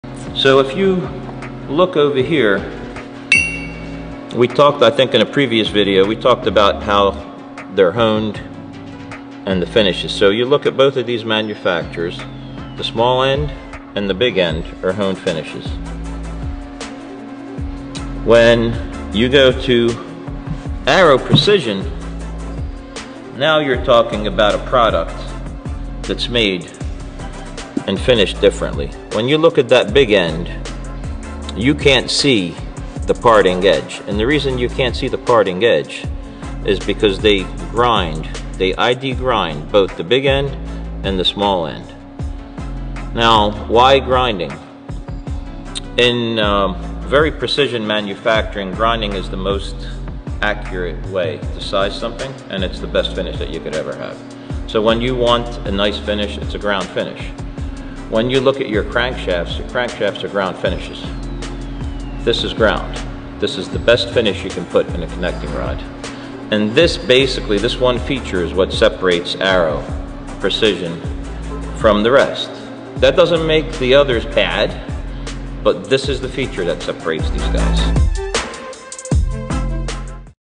BMW Product Comparison | CP-Carrillo vs. Arrow Precision Connecting Rods
S55 vs. S58: join us as we take a closer look at some differences between aftermarket performance connecting rods for the Bavarian bruisers!
While the factory components are quite stout off the showroom floor, even the stock rotating assembly has its limits at significantly increased boost levels.
From basic bolt-on parts to complete race engines, we have every level of performance covered for your S55, S58, or any other BMW powerplant!